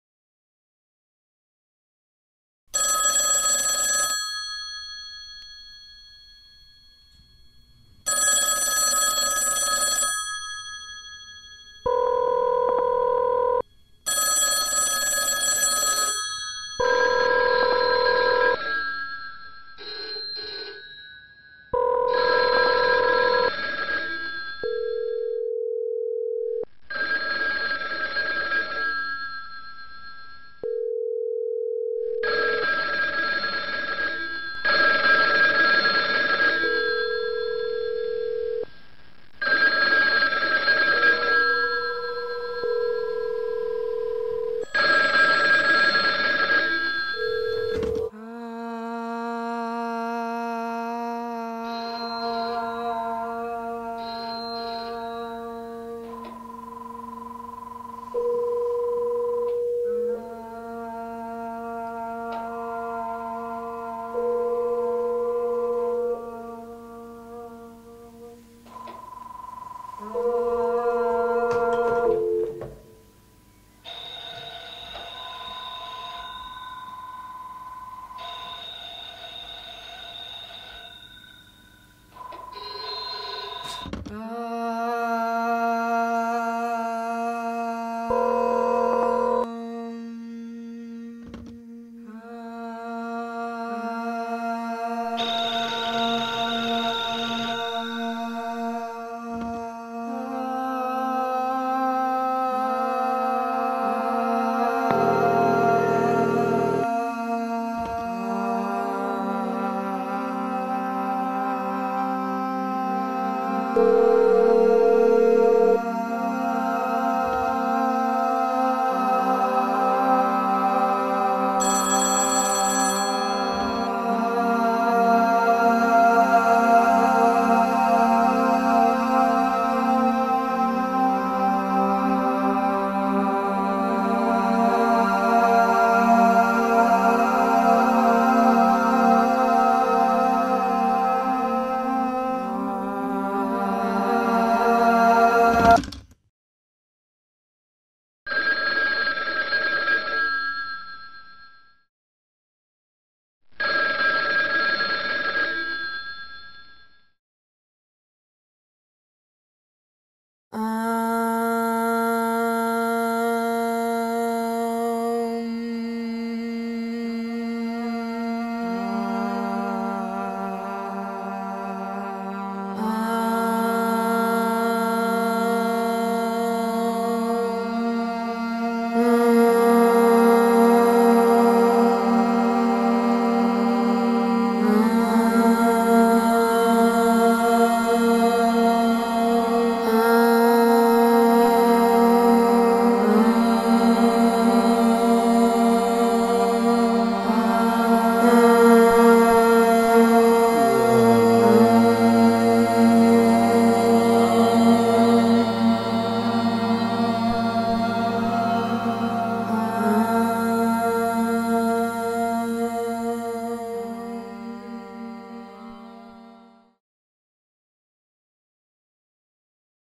O telephone (sound installation)
O telephone
8 channel interactive sound installation, 6 x 6m
Don Ritter, 2007

Six modified 1960’s telephones are arranged in a circle within a darkened room. Each randomly rings with a distinctive sound. If a viewer answers a ringing phone, “om” is heard through the handset and through the speaker in the body of the phone. When viewers answer other ringing phones within the installation, the resulting “om” sounds will pan through all the answered phones. If no ringing telephones are answered by viewers, the telephones will spontaneously begin a new composition comprised of “om” sounds. If viewers pick up a telephone at this time, all sounds will stop and the phones will eventually start ringing again.